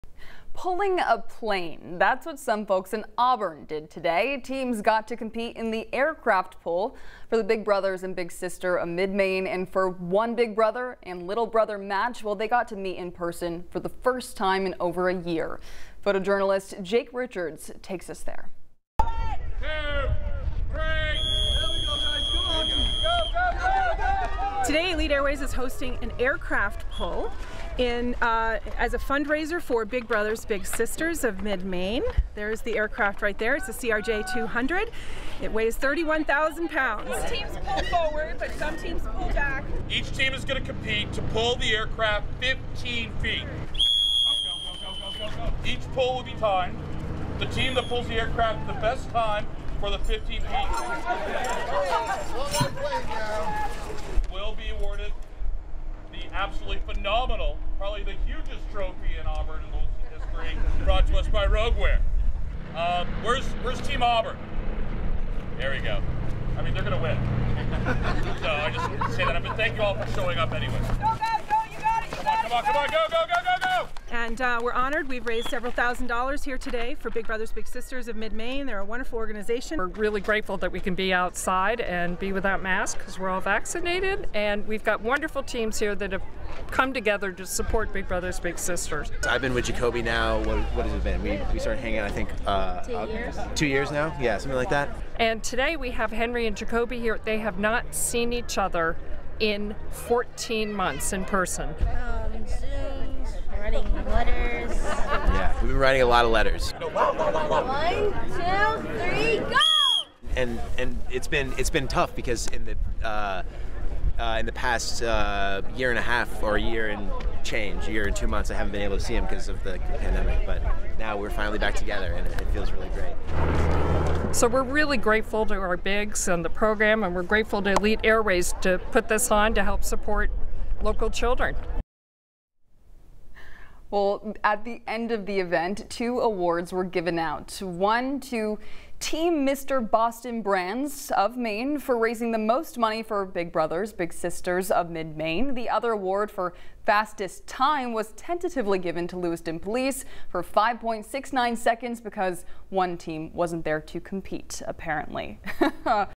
Teams compete in 'Aircraft Pull' for Big Brothers Big Sisters of Mid-Maine
The event raised several thousand dollars for the organization.